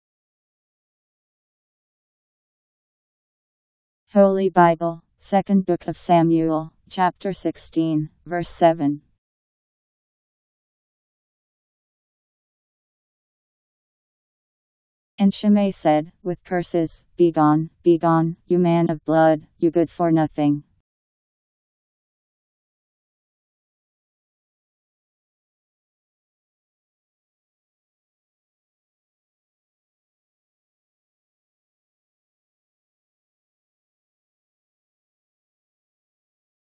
Holy Bible Second Book Of Samuel 16:7
Video shows Holy Bible Second Book Of Samuel Chapter 16 Verse 7: And Shimei said, with curses, Be gone, be gone, you man of blood, you good-for-nothing: Powered by MaryTTS. Text taken from Bible in Basic Edition. Bible Second Book Of Samuel 16:7